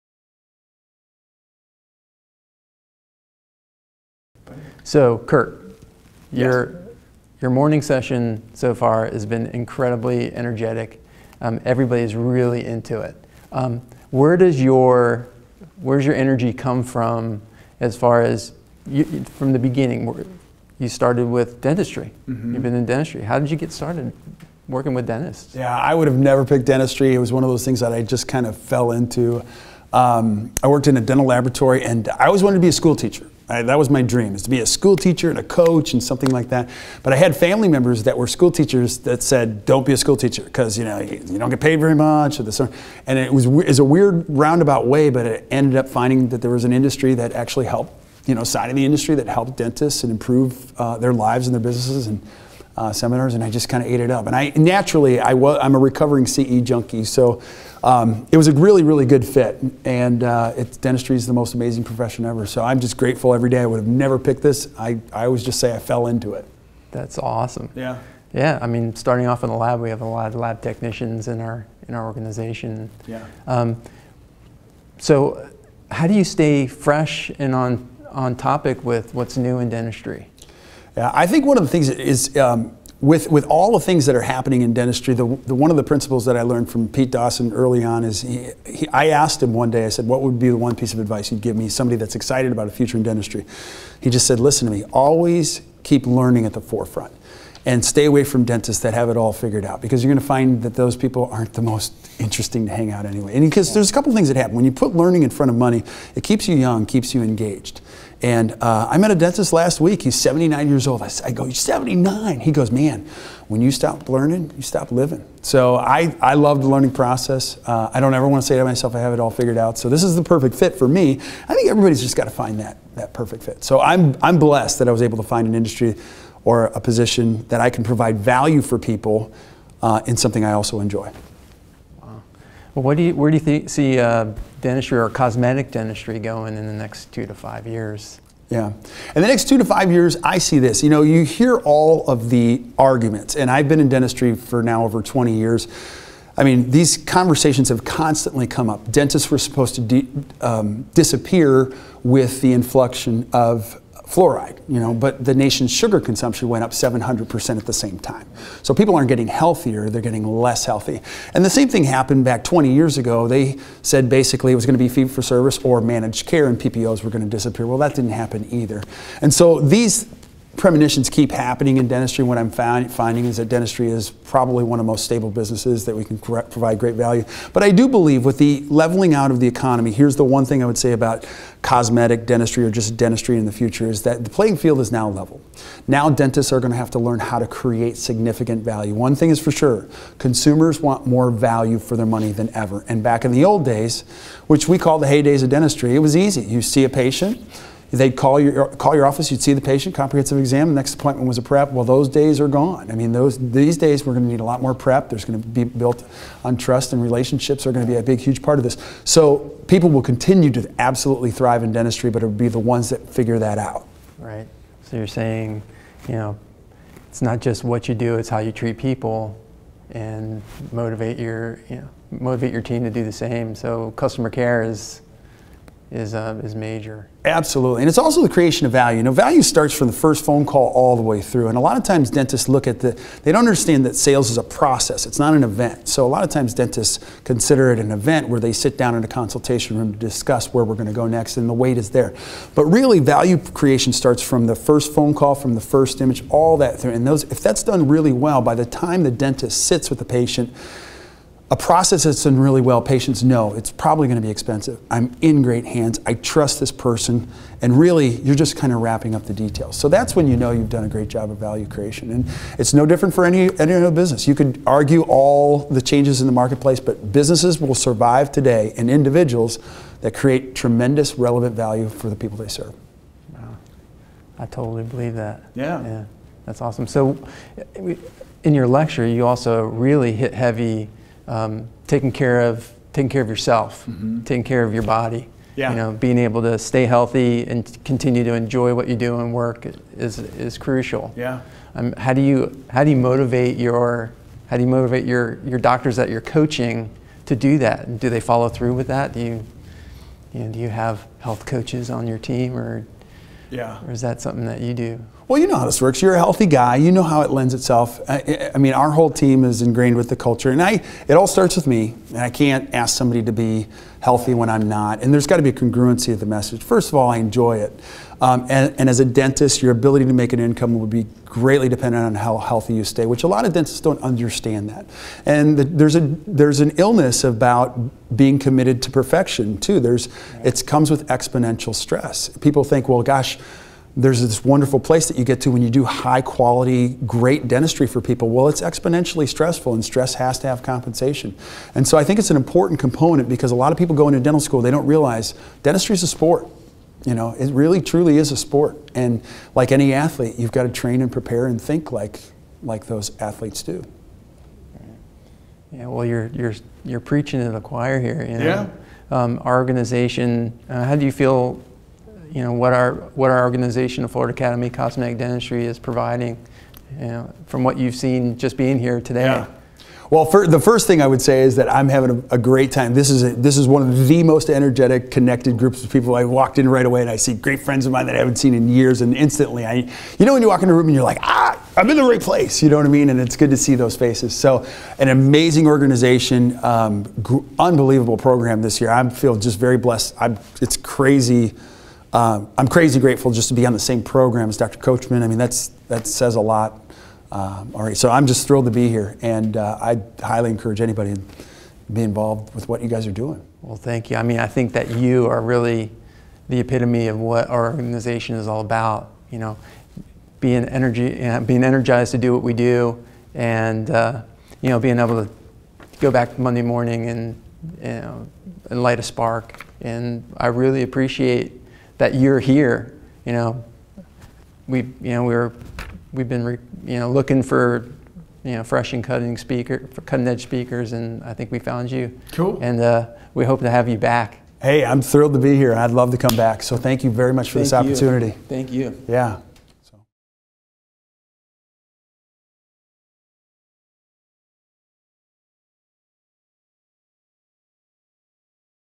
Kurt Behrendt Interview at FACD 2014 Meeting
Dr. Brian Olitsky interviews Kurt Behrendt at FACD 2014